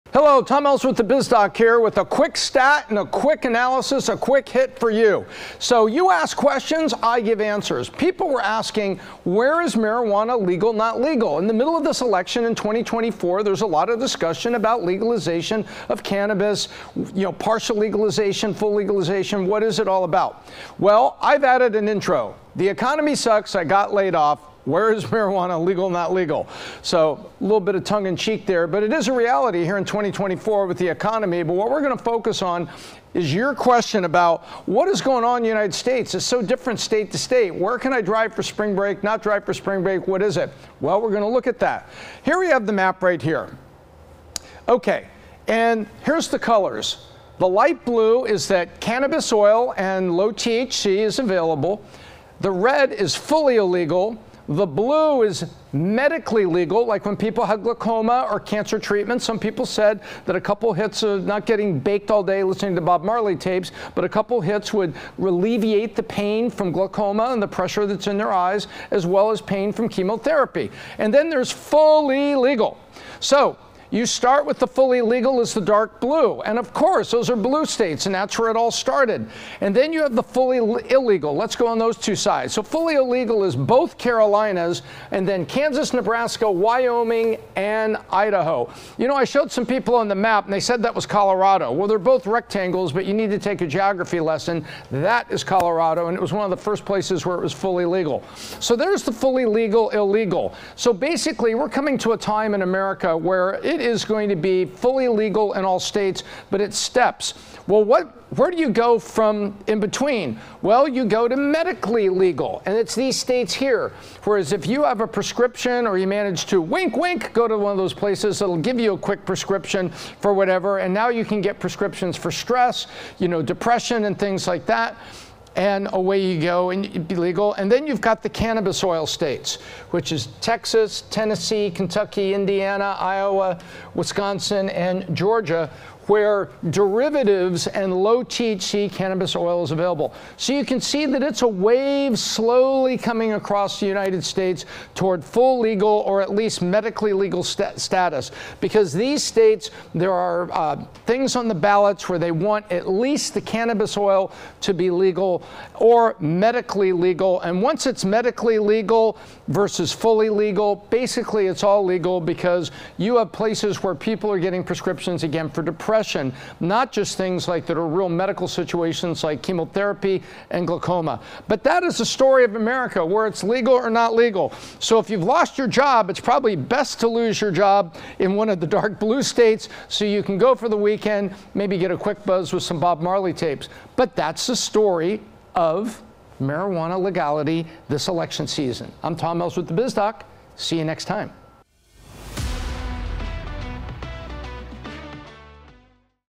Where the Grass is Greener: A State-by-State Guide to Marijuana Legality | Ask The Doc
Amidst the tumultuous economic landscape where pink slips fly like confetti, one burning question prevails: where can you legally puff away your job woes? Join us on a journey through the green-tinged corridors of legality, where the only pink slips you'll find are the ones holding your rolling papers.

Watch The Biz Doc Podcast every Friday at 1pm & catch a new "Biz Doc Bite" every single day at 9:30am EST.

And I hope I left you better than I found you.